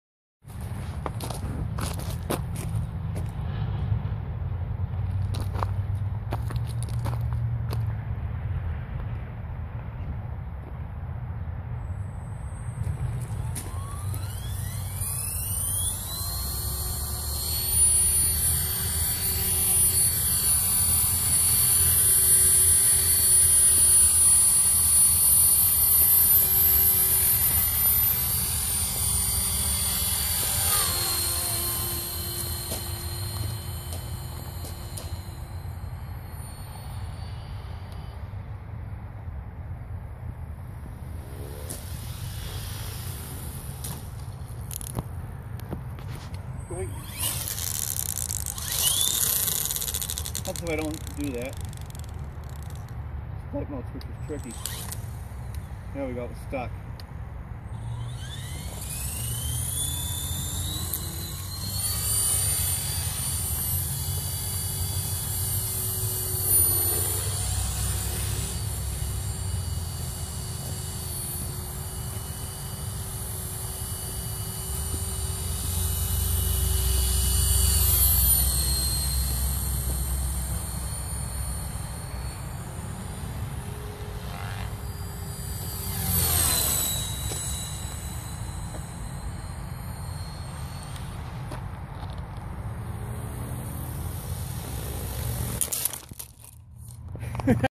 Dynam E-razor Stock Auto Rotations and Inverted Flying!
Hammering on a completely stock flybarred Dynam E-Razor 450.
I'm not sure why but I've not seen anyone on youtube fly one of these flybarred ones inverted!  This might be the only one!
Everything stock. except the radio and gyro.
OrangeRX 6CH receiver and GA-250 $10 mems HobbyKing Gyro.
This is the carbon edition airframe.
Plastic one flies the same.